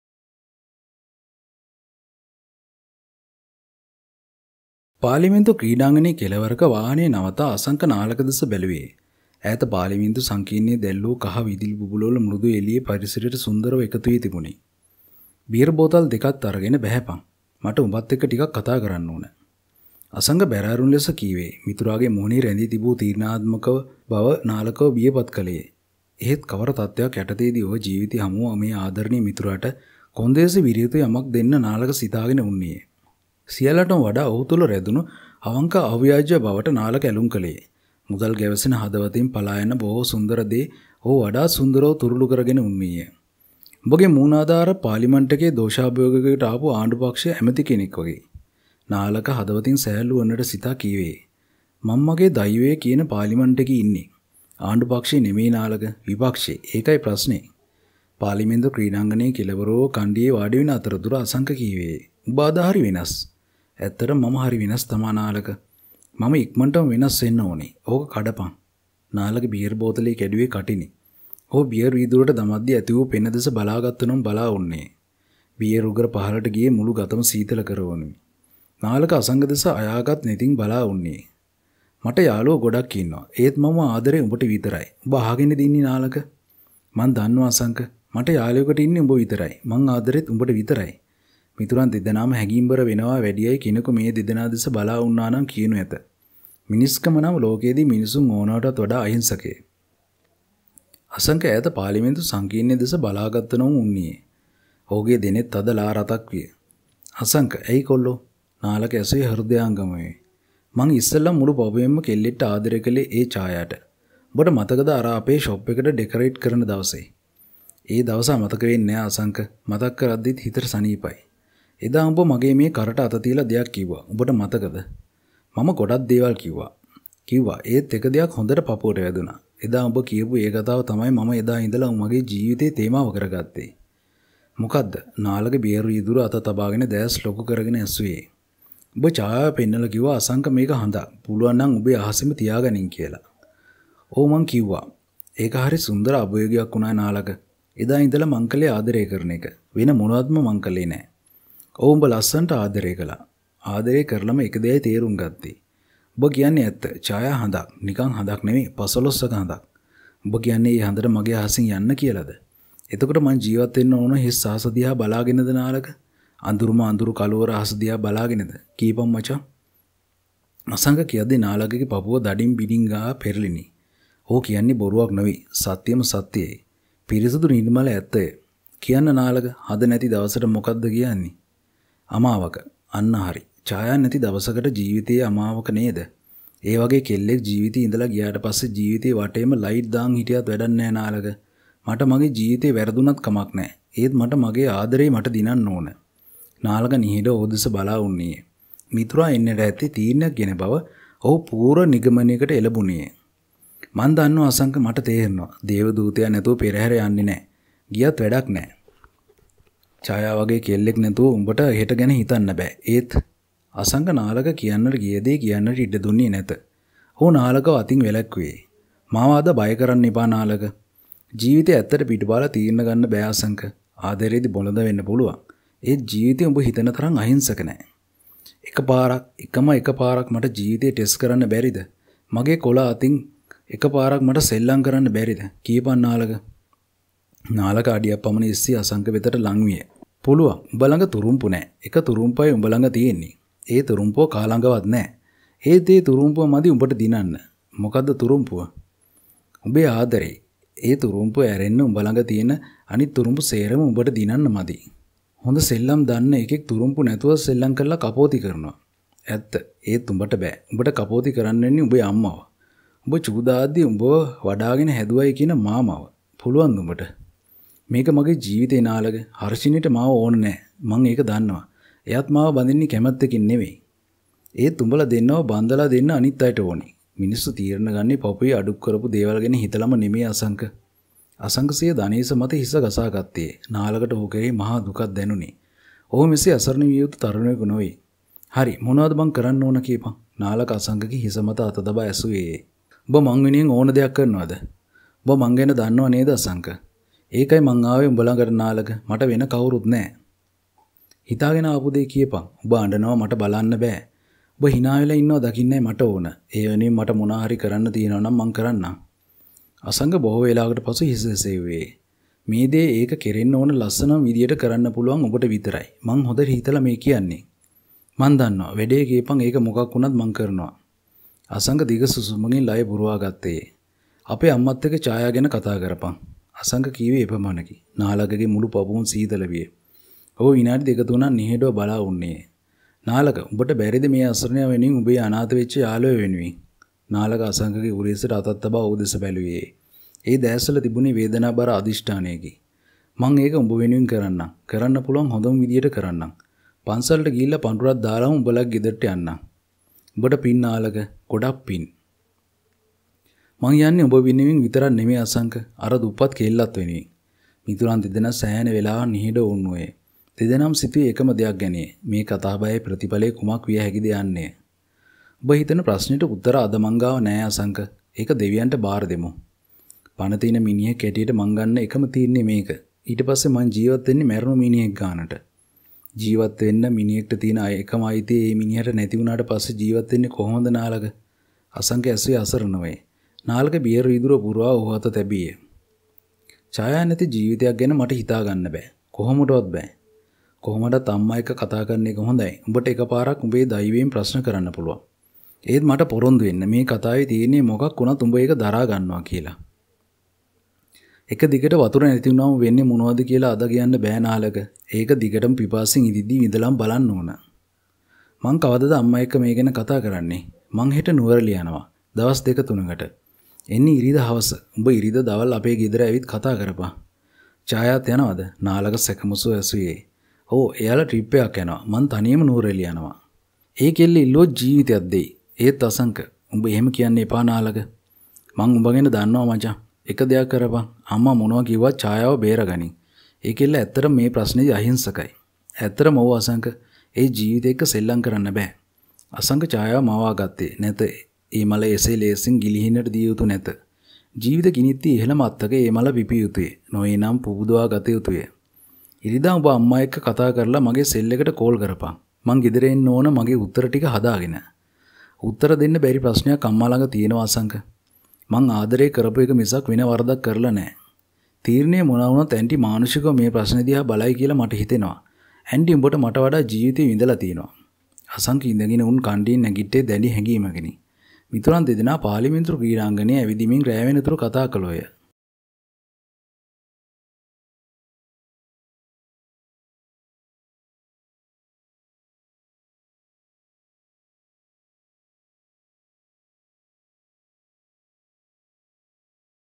😊 චායා - Chaya 61 ❤ | එදා ඉඳලා මං කළේ ආදරේ කරන එක. | Vish Novels | Sinhala Novels
කතාවෙන් බිඳක්: ඵ්ක හරි සුන්දර අභියෝගයක් වුණා නාලක... එදා ඉඳලා මං කළේ ආදරේ කරන එක. වෙන මොනවත්ම මං කළේ නෑ.
ඔව්... උඹ ලස්සනට ආදරේ කළා...
ආදරේ කරලම එක දෙයයි තේරුම් ගත්තේ. චායා හඳක්... නිකම් හඳක් තෙමේ පසළොස්වක සඳක්... උඹ කියන්නේ ඒ හඳට මගේ අහසින් යන්න කියලද... එතකොට මං ජීවත් වෙන්න ඕන හිස් අහස දිහා බලාගෙනද නාලක. අඳුරුම අඳුරු කළුවර අහස දිහා බලාගෙනද. කියපත් මට.
අසංක කියද්දි නාලකගේ පපුව දඩි බිඩිං හා පෙරළිනි. ඔහු කියන්නේ බොරුවක් නොවේ.

A bit of the story: It was a beautiful challenge Nalaka... Since then I have been making love. I didn't do anything else.
Yes... you loved beautifully...
I understood only one thing after falling in love. a chaya moon... just a moon, a crescent moon... are you telling that moon to leave my sky... then do I have to live looking at the empty sky or in a canal. the darkest Are you looking at the dark sky. tell me.
Nalaka's chest heaved and heaved while saying Asanka. He is not lying.

😊 චායා - Chaya 61 ❤ | එදා ඉඳලා මං කළේ ආදරේ කරන එක.  | VishwaNovels | Sinhala Novels

Time Codes
0:00 - Intro
0:11 - Love Story
07:55 - End